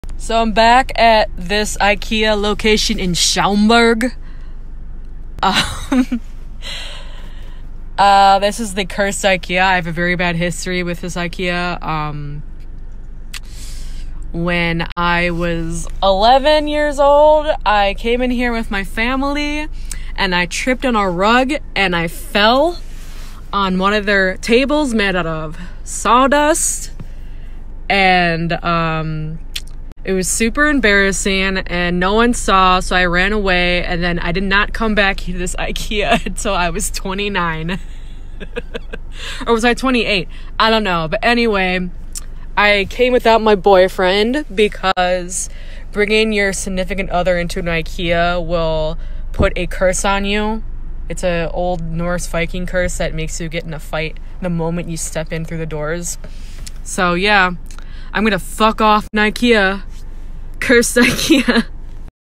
Cursed IKEA
Story about why I went to this IKEA when I was 11 years old and didn’t return until I was 28.